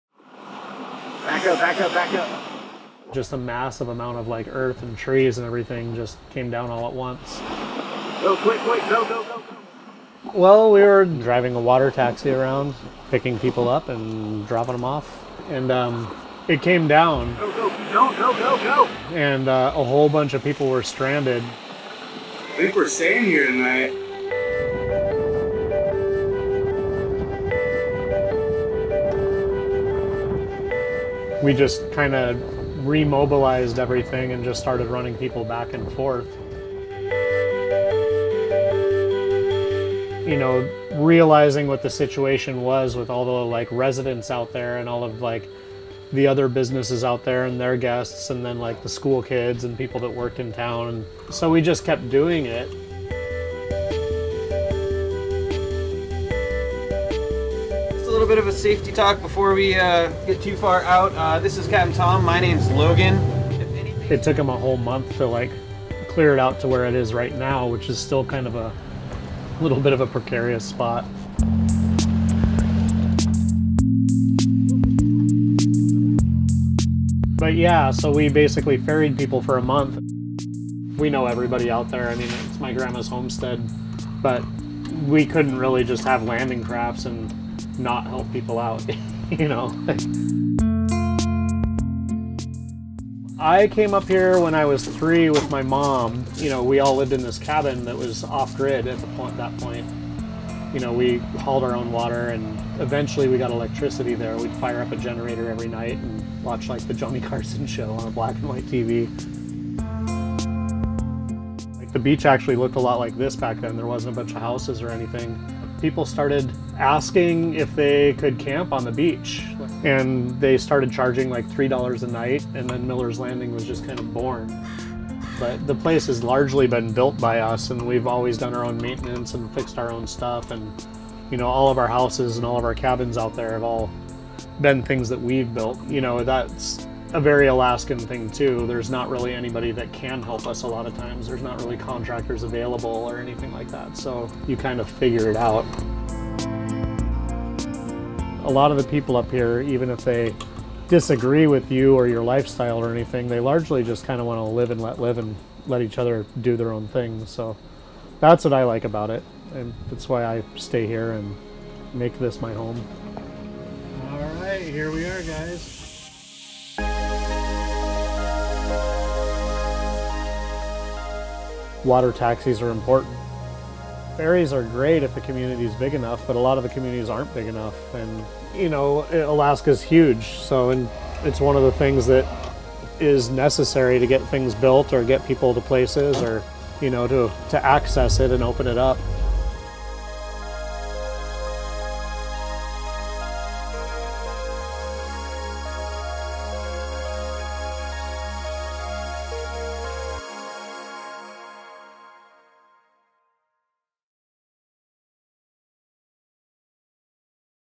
The important role of water taxis in Alaska | INDIE ALASKA
When a massive, unexpected landslide blocked the only road to Lowell Point, Tom Miller knew his water taxi service was the only way to move residents, school children, and tourists back and forth to the nearby town of Seward. With roughly 6,640 miles of coastline, water taxis are a vital part of the transportation and infrastructure in Alaska. In this episode of Indie Alaska, we hear Tom's story and the history of his family's business, Miller's Landing.

Video by Valerie Kern
Video and Story by Mizelle Mayo
Landslide video courtesy of Patrick Higgins

Consider supporting Alaska Public Media and ensure storytelling like INDIE ALASKA continues into the future.